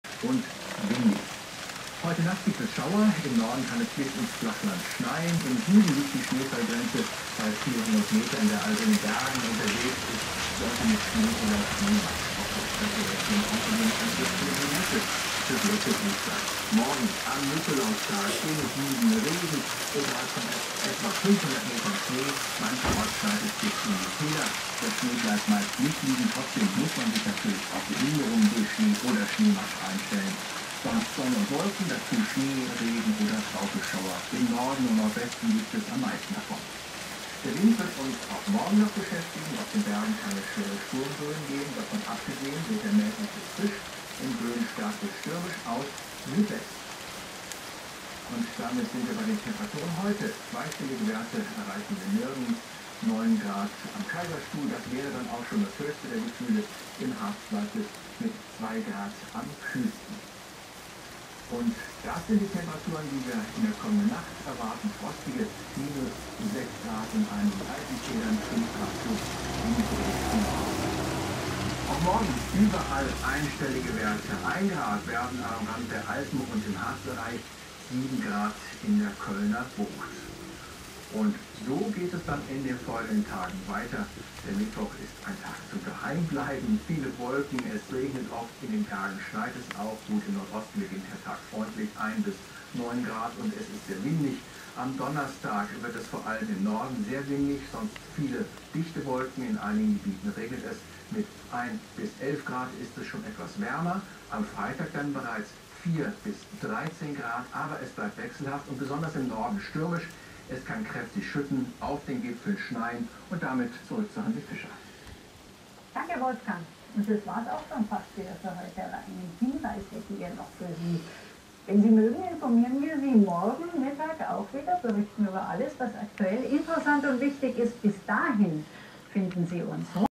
Der erste Schnee 2011-12
Montag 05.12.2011 Mittag 14 Uhr Schnee mit Blitz und Donner hab ich aber verpasst
und er ist schon wieder weg